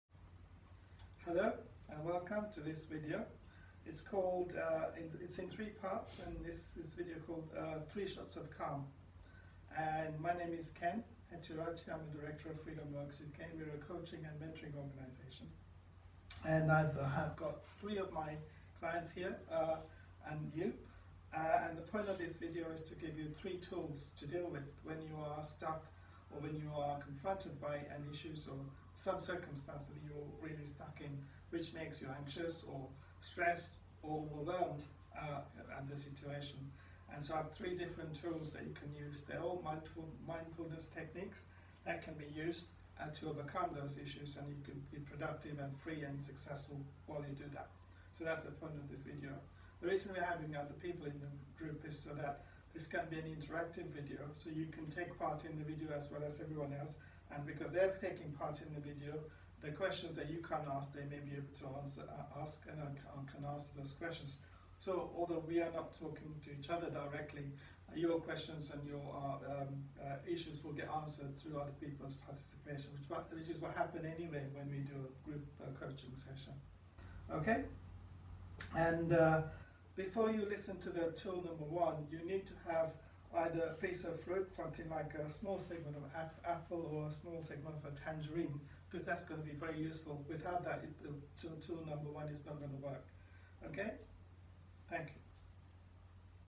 Freedom Works UK - 3 Shots of calm
Freedom Works UK is a coaching and mentoring organisation and this is the introduction to our coaching tool - 3 shots of calm from stress, anxiety & overwhelm.